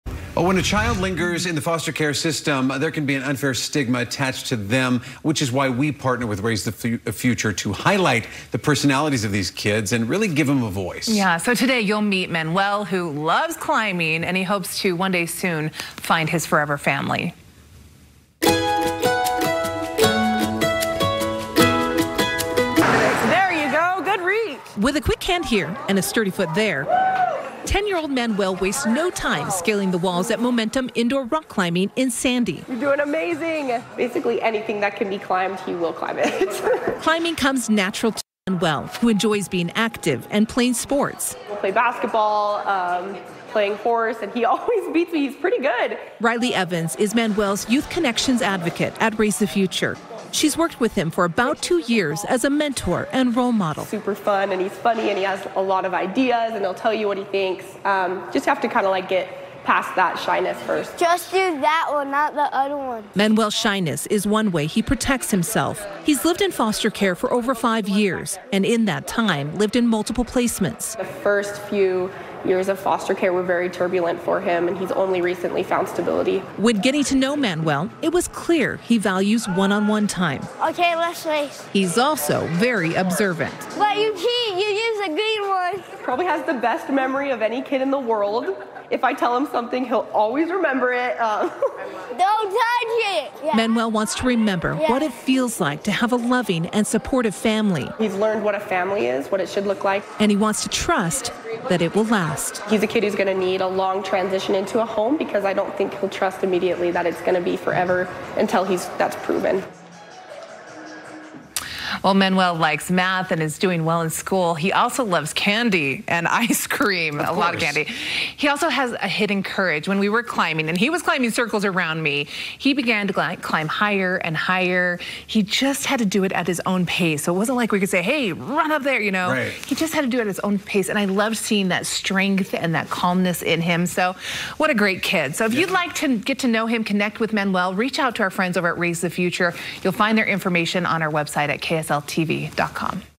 Wednesday's Child: Manuel, 10, loves climbing; hopes to be adopted by an active, loving family
Manuel, 10, enjoys being active and playing sports. He's been living in foster care for more than five years and says he wants to remember what it feels like to have a loving and supportive family.